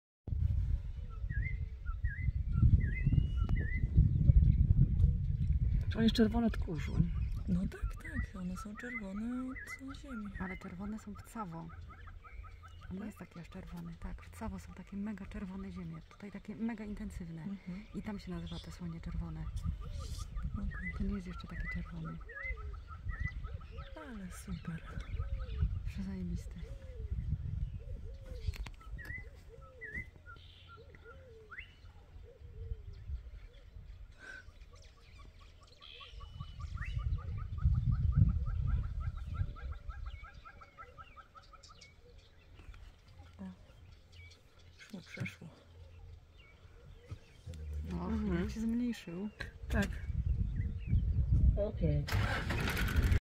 Elephant Mock Charging a Safari Vehicle
Many thanks to @co_romeo for the video!

Location:Kenya